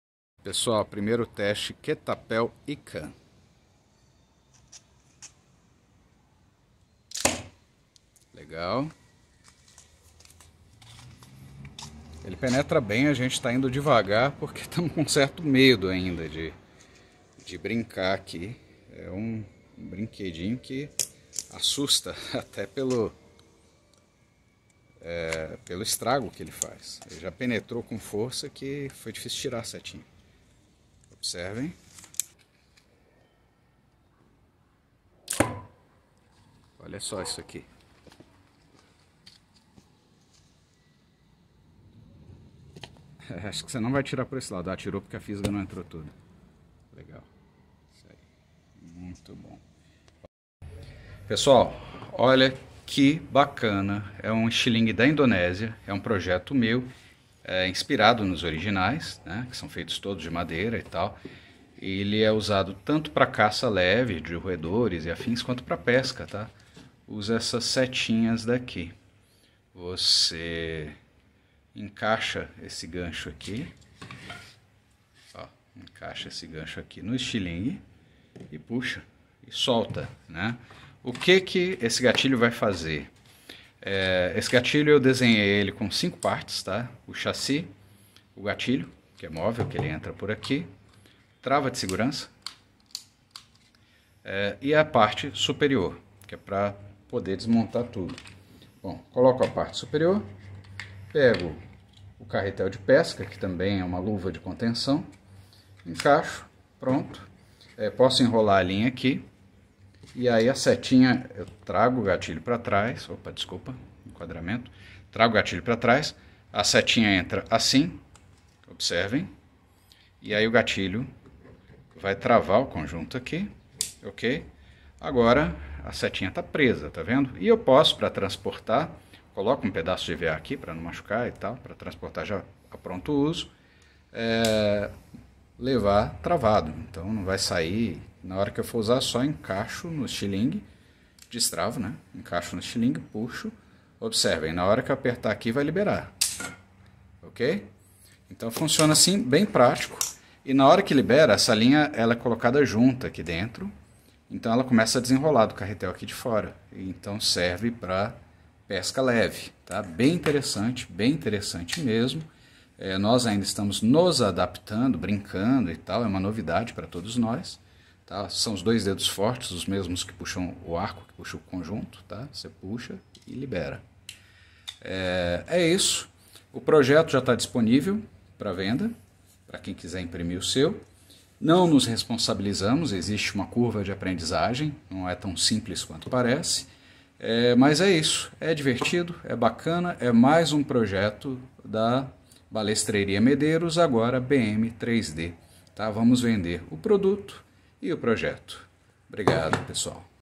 Estilingue de pesca da Indonésia
Gatilho para estilingue de pesca inspirado no modelo da Indonésia "Membuat Ketapel Ikan"